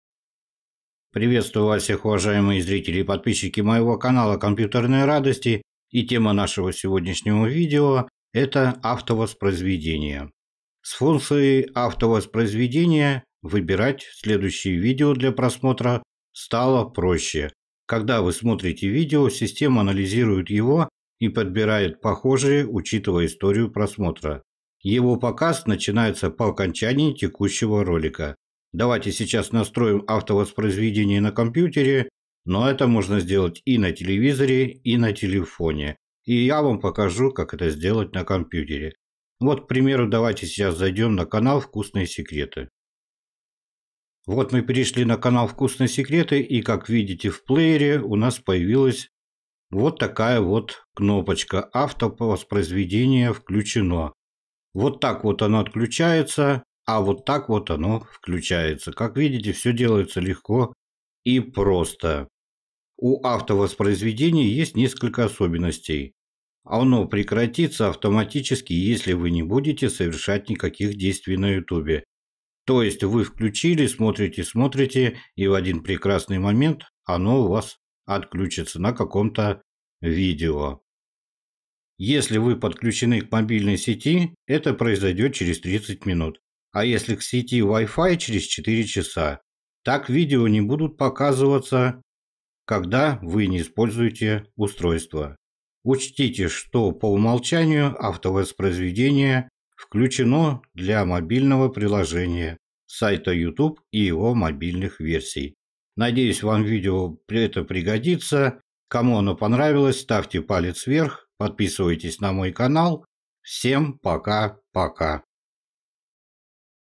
Автовоспроизведение видео YouTube
Автовоспроизведение видео на YouTube, что это такое? Постараюсь раскрыть весь материал в этом видео! Затрону темы, как отключить автовоспроизведение навсегда и включить автовоспроизведение на ютубе.